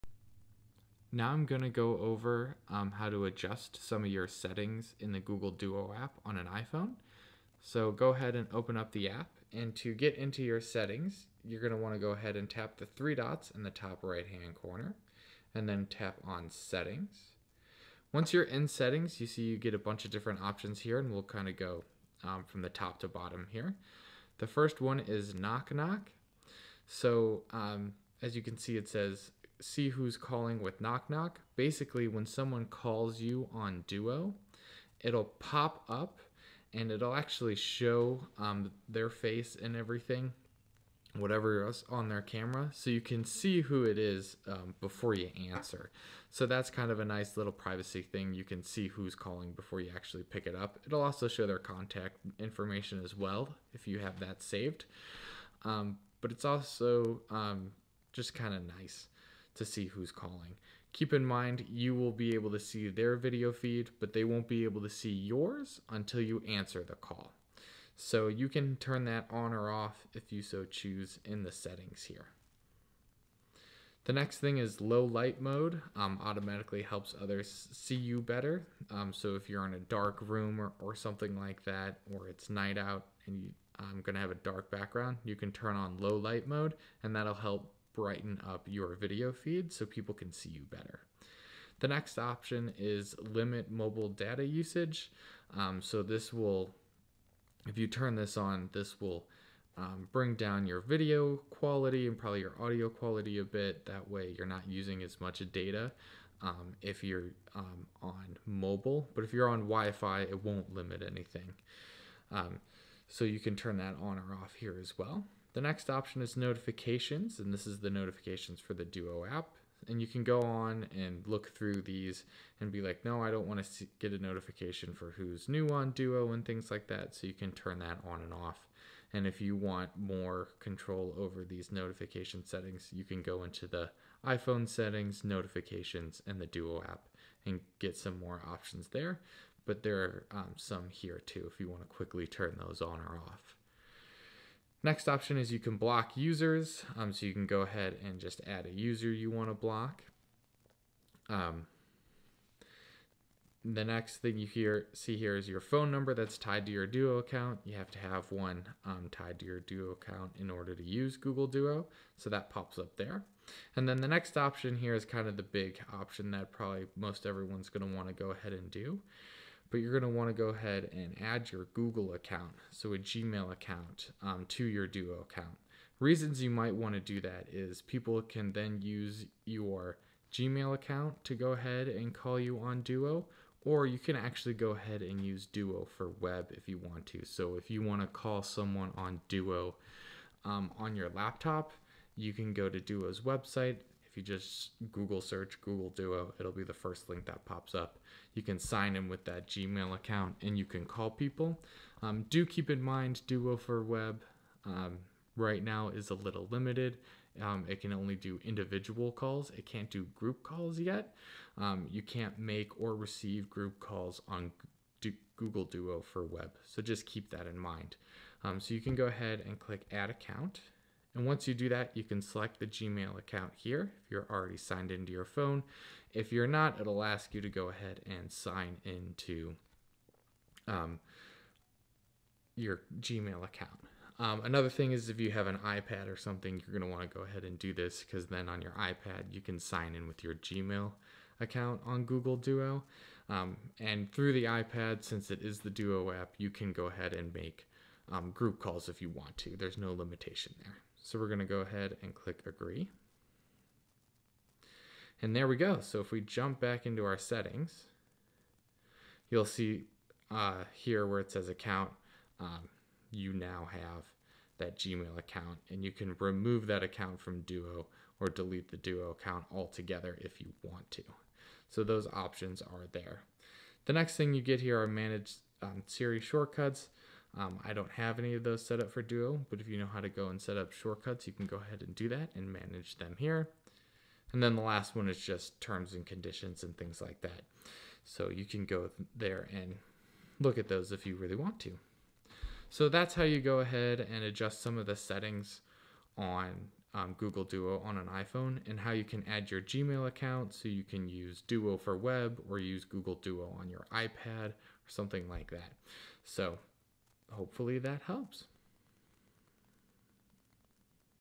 Duo: iPhone: Add Gmail account and adjust settings
How to adjust your Duo settings on an iPhone. We also go over how to connect your Gmail account to your Duo account. This way you can download and us the Duo app on your iPad. To use Duo on your iPad you need to sign in with your Gmail so make sure you connect those accounts first. Also you will be able to use Duo for web if you connect your accounts.

At this time Duo for web only allows you to make and receive one on one video calls. If you want to do a group Duo call you, and everyone else, will have to use the Duo app on iPhone, iPad or Android. As long as everyone in the group has one of those devices then you will be good to go!

How to setup Duo on an Android device: